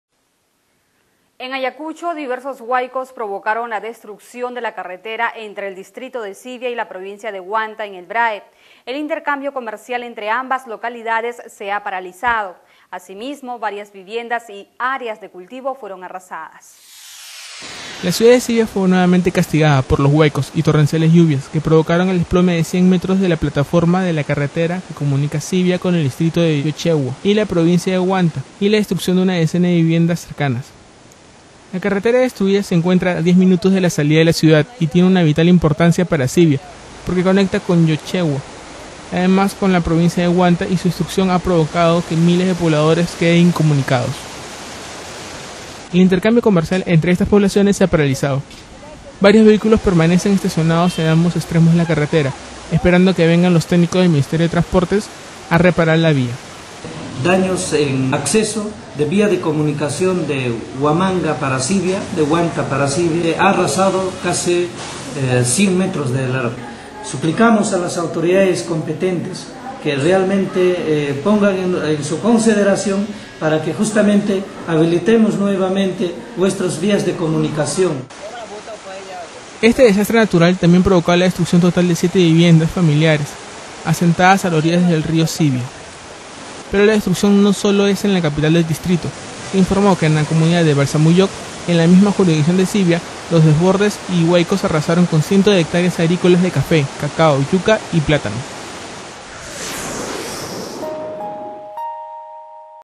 Ayacucho: Huayco arrasa cultivos y destruye carretera entre Sivia y Huanta
La ciudad de Sivia fue nuevamente castigada por los huaycos y torrenciales lluvias que provocaron  la destrucción de una decena de viviendas cercanas y el desplome de cien metros de la plataforma de la carretera que comunica Sivia con el distrito de Llochegua y con la provincia de Huanta. La carretera destruida se encuentra a 10 minutos de la salida de la ciudad y tiene una vital importancia para Sivia porque la conecta con LLochegua y además con la provincia de Huanta. Su destrucción ha provocado que miles de pobladores queden incomunicados.
El intercambio comercial entre todas estas poblaciones se ha paralizado. Varios vehículos permanecen estacionados en ambos extremos de la carretera esperando que vengan los técnicos del MTC a reparar la vía.
Ahmed Bermuda, responsable de la oficina de información del Municipio de Sivia, informó que se han producido daños en las vías de comunicación de Huanta para Sivia. Este desastre natural también provocó la destrucción total de siete viviendas familiares, asentadas en la orilla del rió Sivia.
Pero la destrucción no solo es en la capital del distrito. Se informó que en la comunidad de Valsamullucc en la misma jurisdicción de Sivia, los desbordes y huaycos arrasaron con cientos de hectáreas agrícolas de café, caco, yuca y plátano.